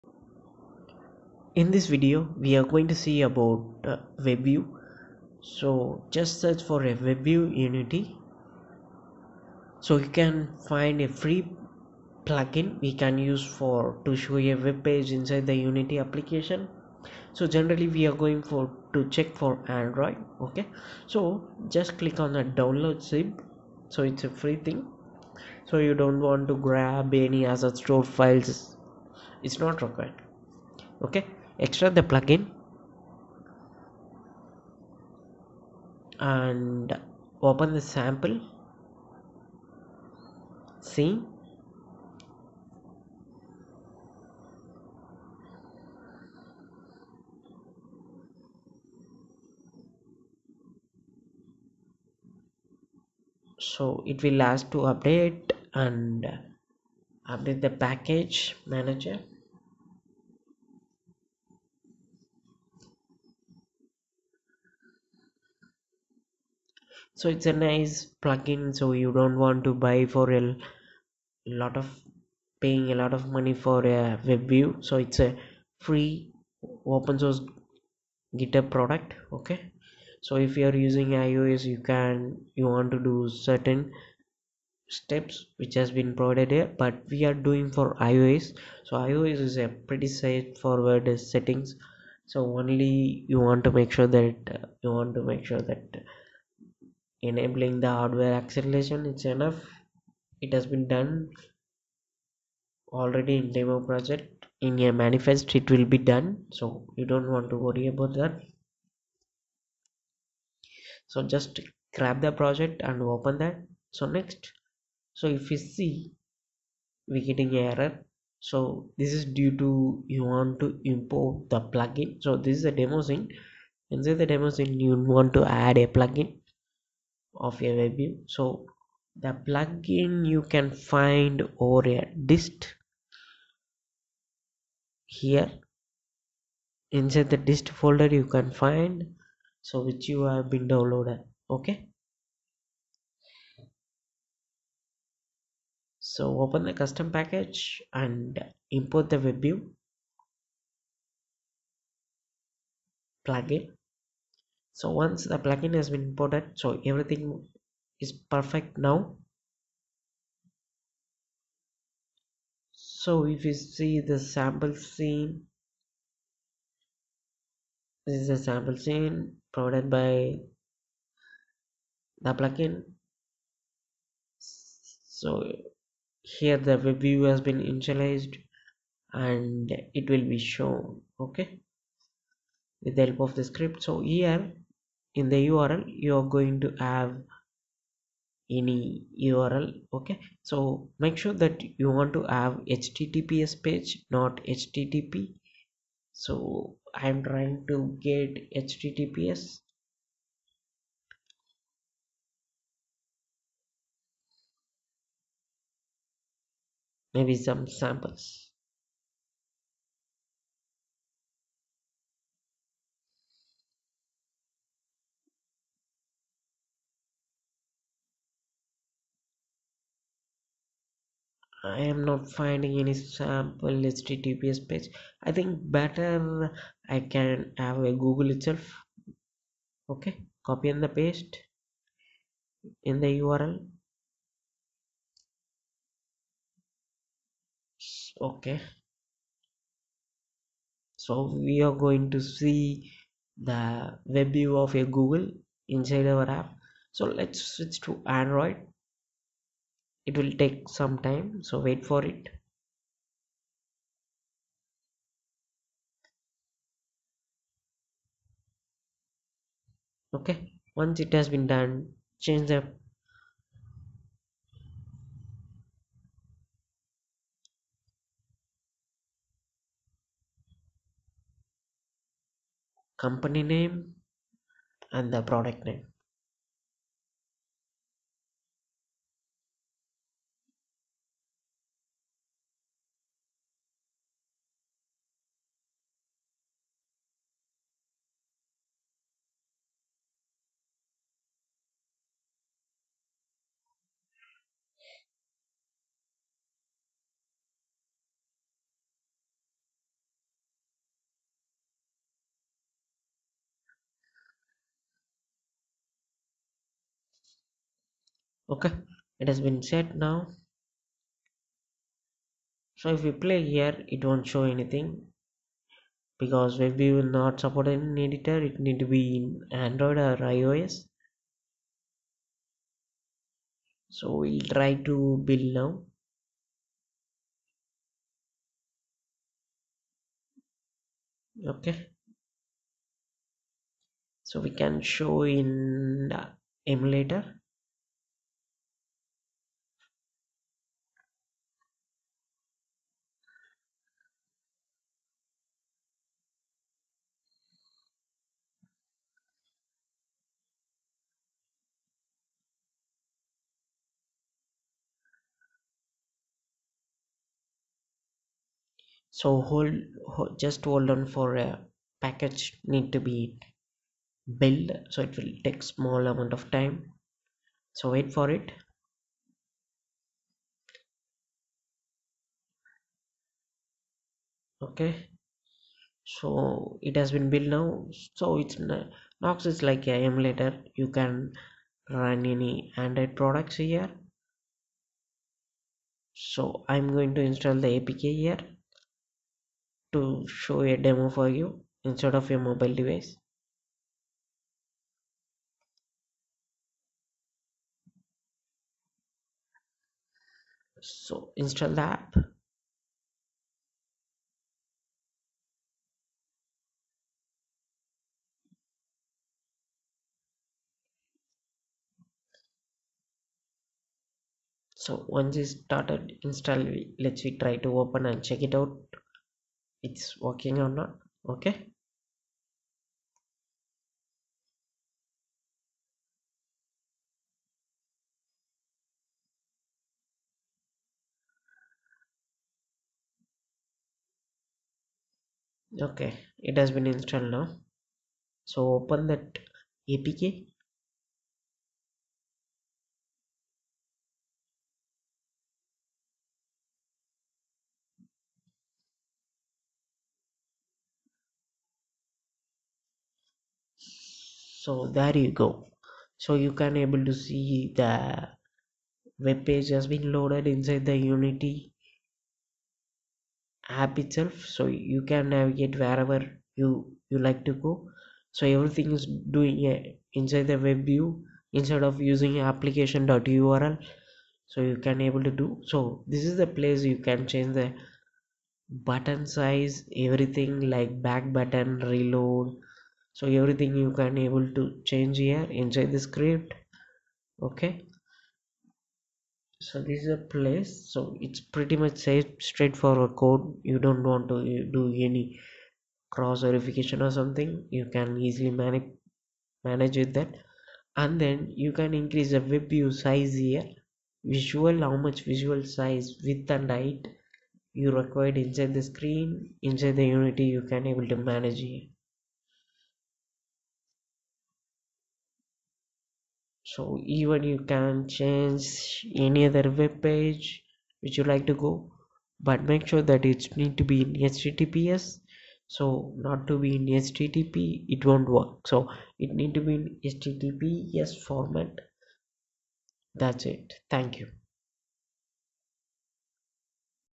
Untiy3d Tutorial: Displaying a webpage inside the unity without leaving the app
using this tutorial you can able to show webpage inside the unity . by without forcing the app to see web page in we browser.

WebView allow to show inside the  app like a html format conversion and shown to the users